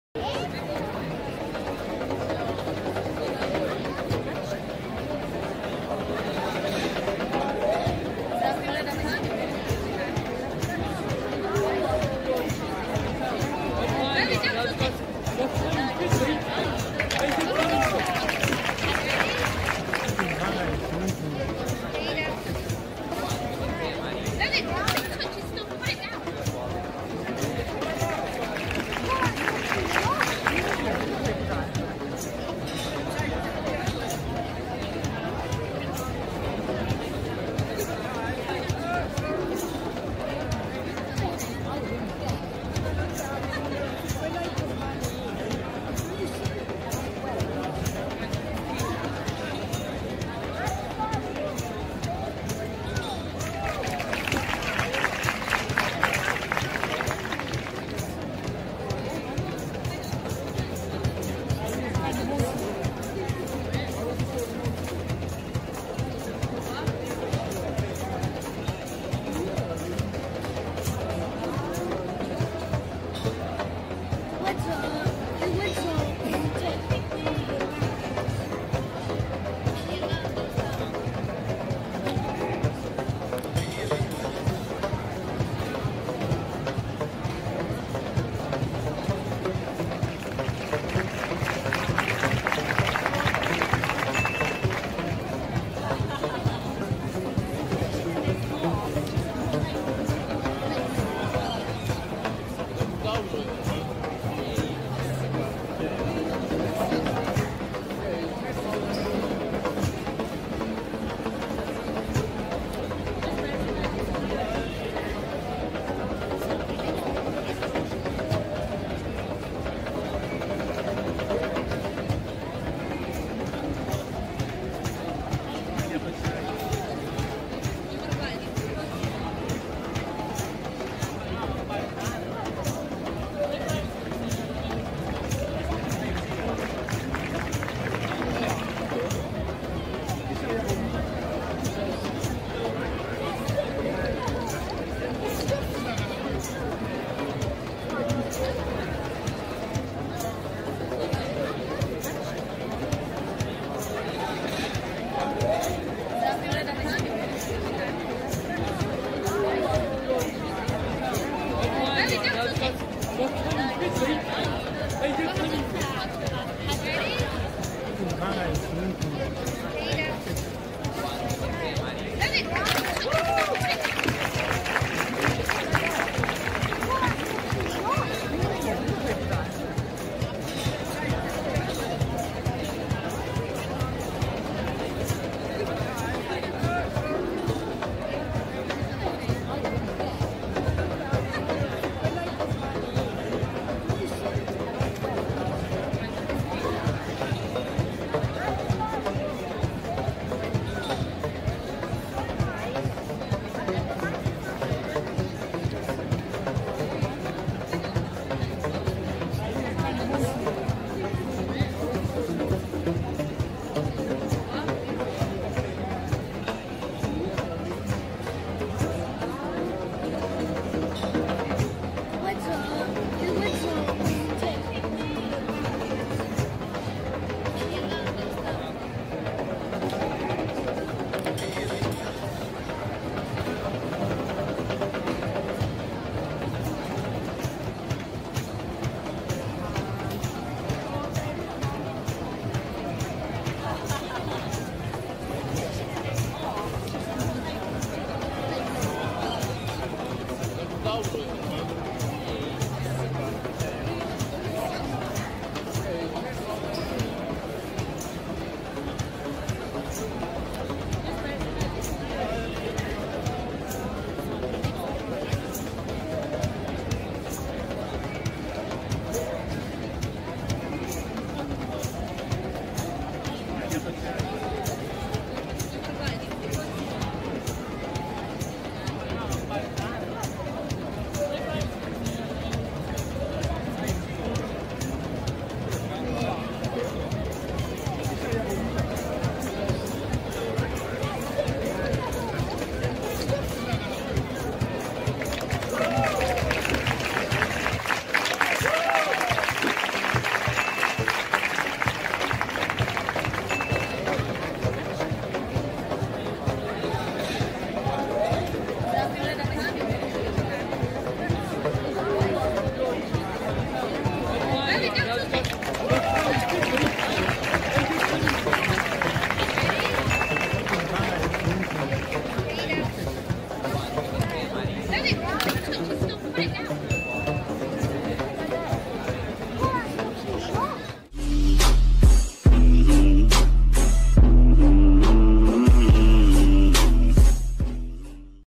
Street Magic
This guy in Leicester Square is absolutely amazing - how he made the mice vanished at the end is amazing, he has been learning magic in India and had taken him months to learn the mice trick :)
Licence